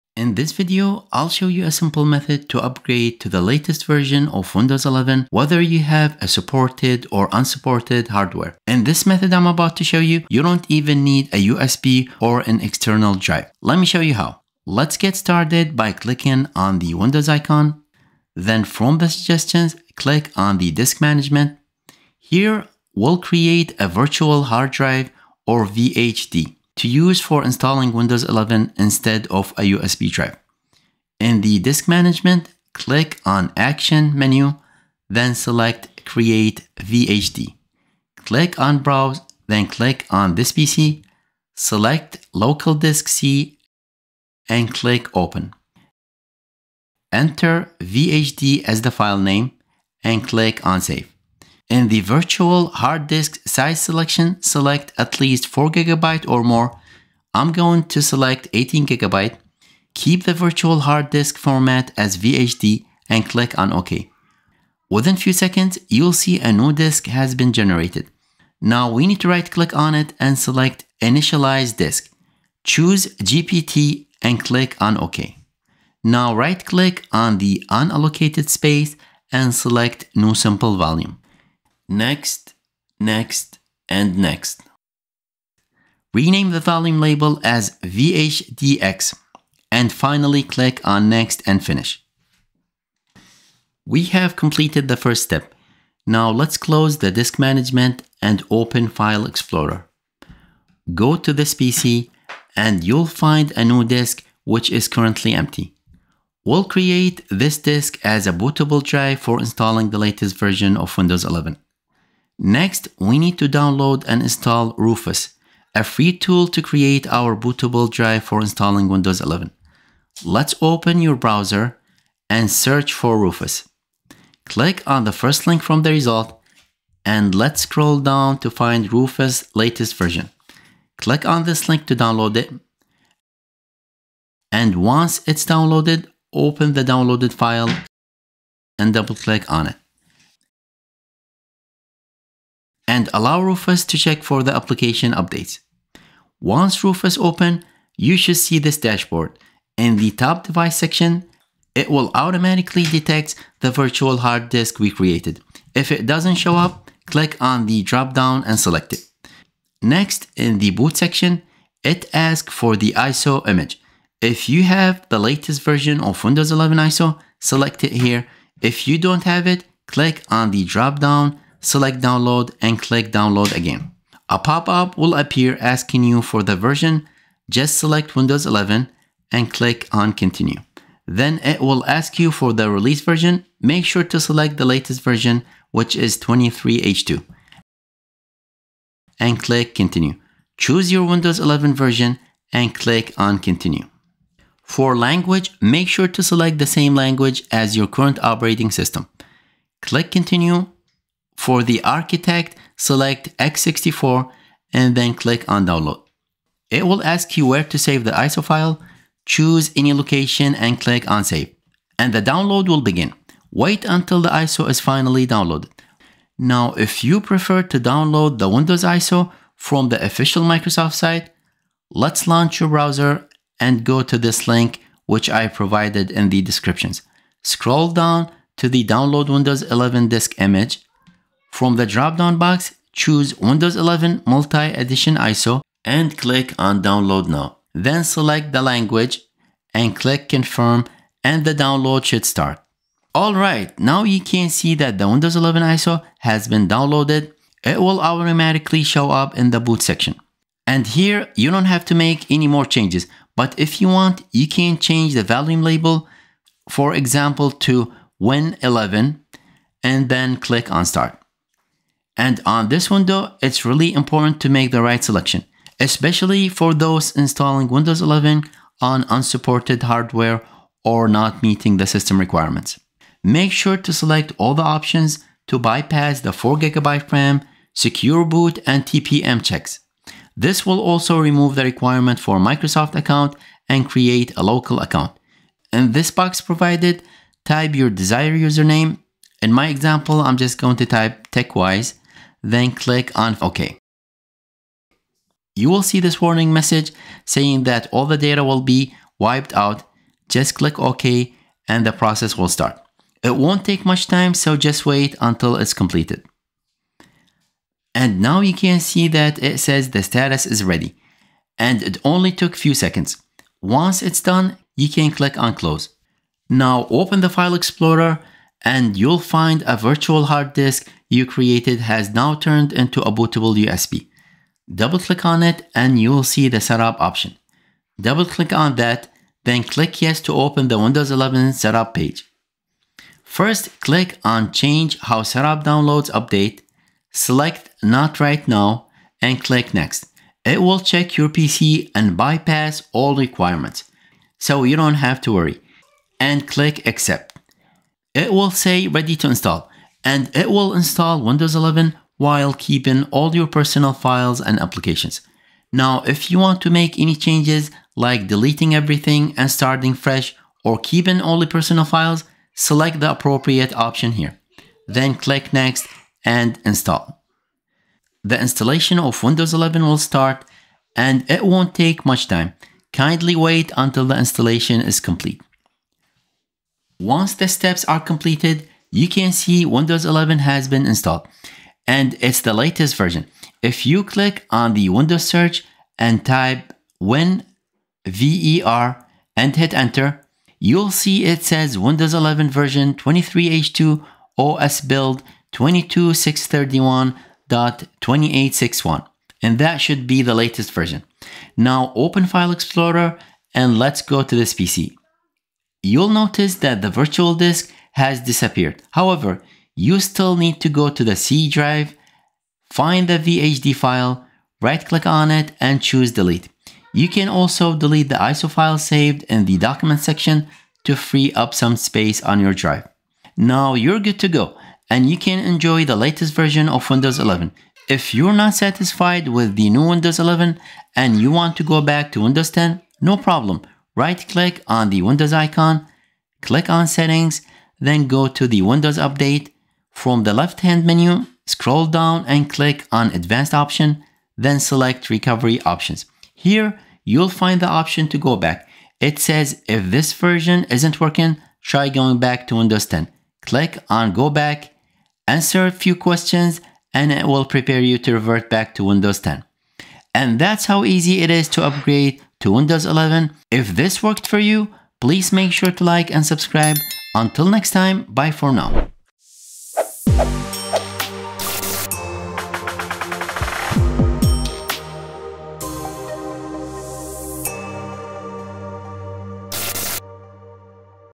How to Upgrade to Windows 11 on Any PC (No USB Needed, Works for Supported & Unsupported Hardware)
In this video, I'll walk you through a simple method to upgrade to the latest version of Windows 11, whether you have supported or unsupported hardware. Best of all, you won't need a USB or external drive for this process.